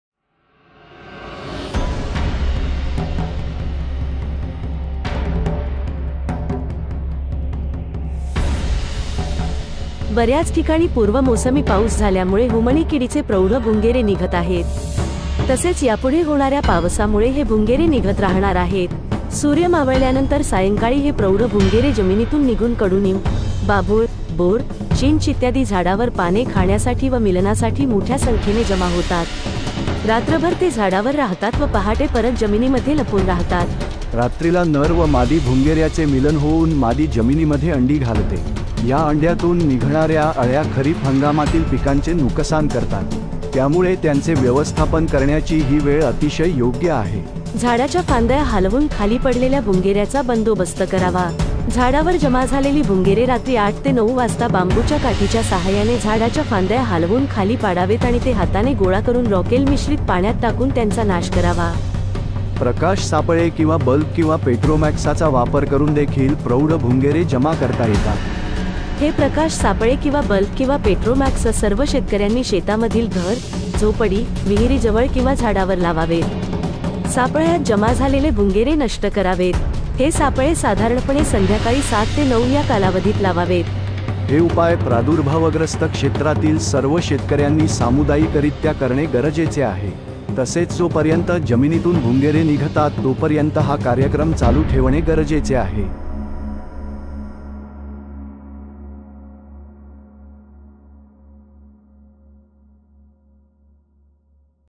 हुमणीच्या प्रौढ भुंगेऱ्याचे व्यवस्थापन White grub adult management
बऱ्याच ठिकाणी पूर्व मोसमी पाऊस झाल्यामुळे हुमणी किडीचे प्रौढ भुंगेरे निघत आहेत. तसेच यापुढे होणाऱ्या पावसामुळे हे भुंगेरे निघत राहणार आहेत. त्यामुळे त्यांचे व्यवस्थापन करण्याची ही वेळ अतिशय योग्य आहे.